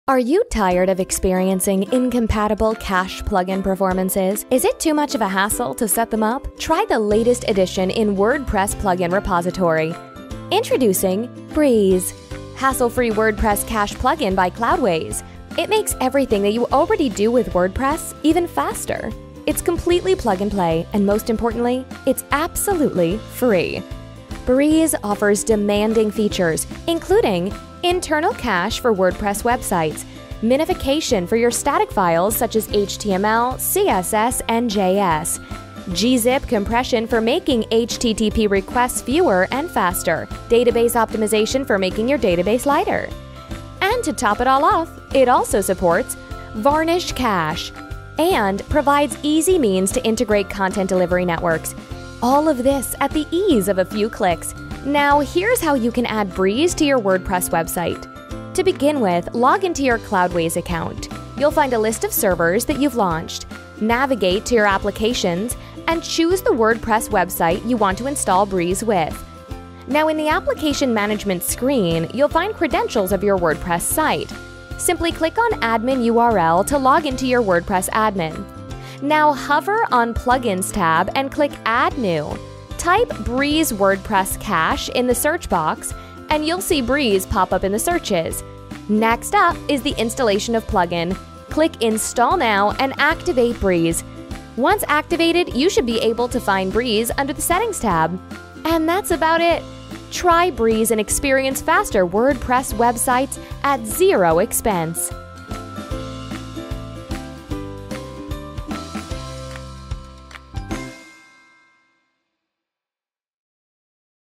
Breeze - Free WordPress Cache Plugin by Cloudways
Breeze is a free, open-source WordPress cache plugin developed by Cloudways.

The plugin has powerful caching, minification, gzip compression, browser cache, CDN integration, Varnish cache support, and database optimization.